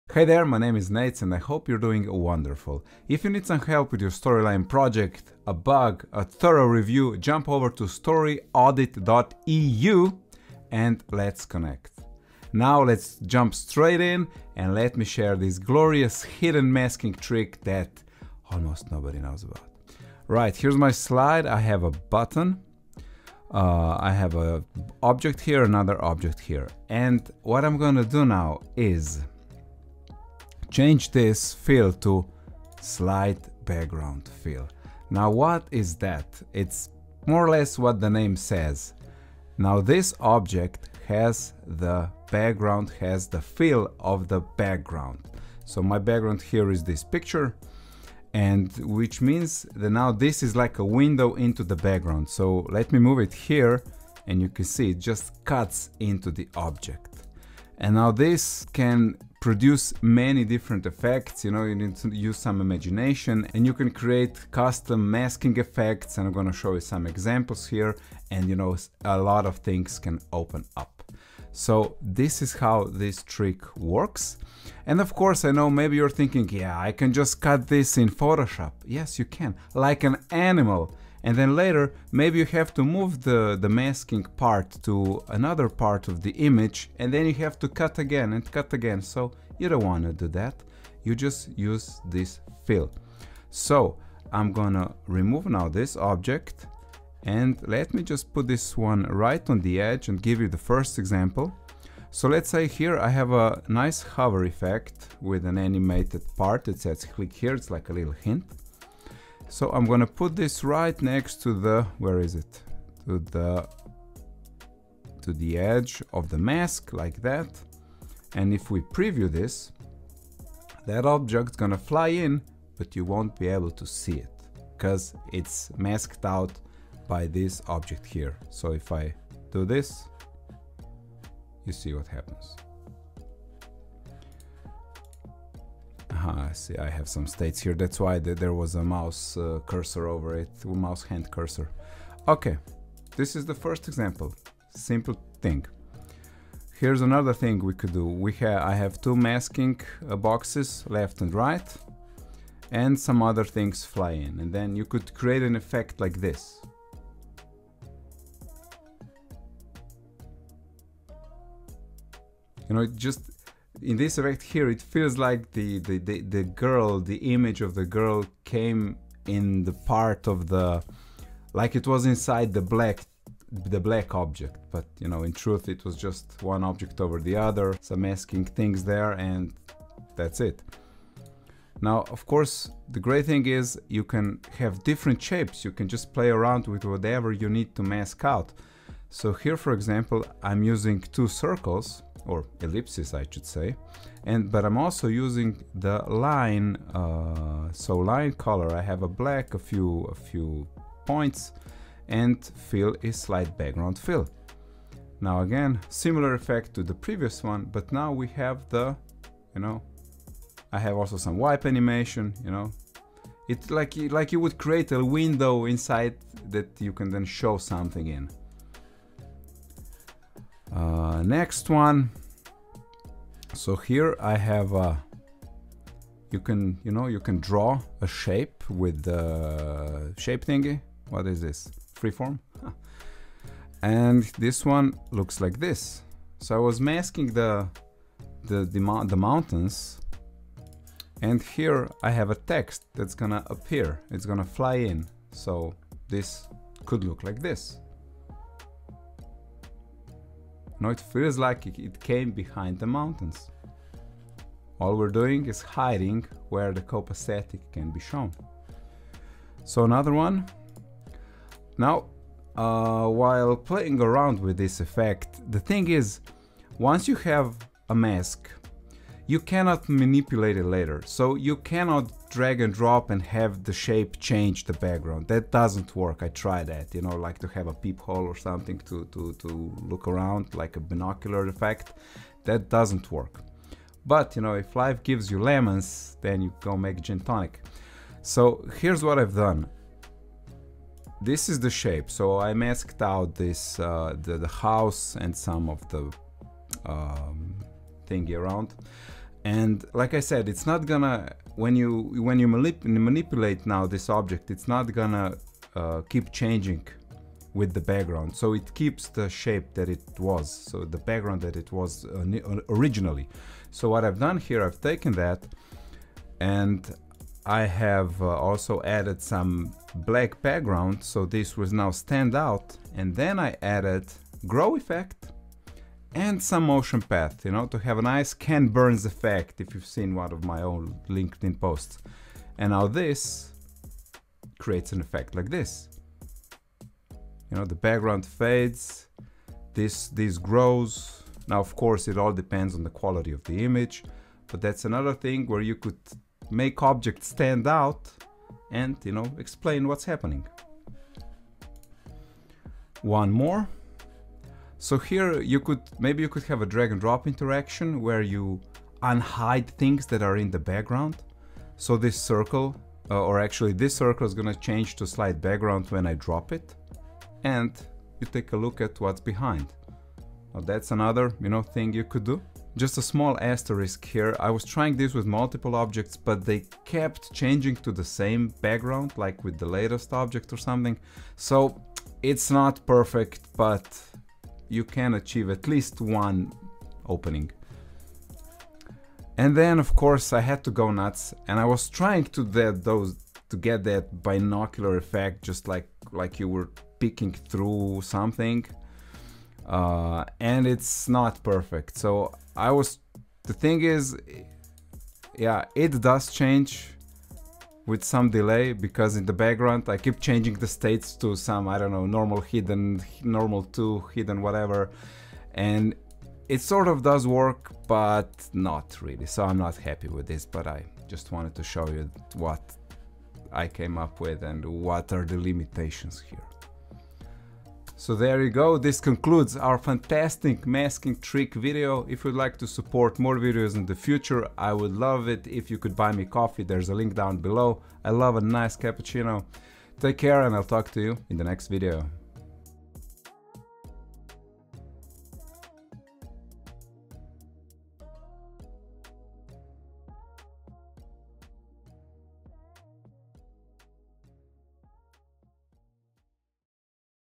Hidden Masking Trick (That No One Knows About)
This hidden masking trick in Articulate Storyline opens up many different design options.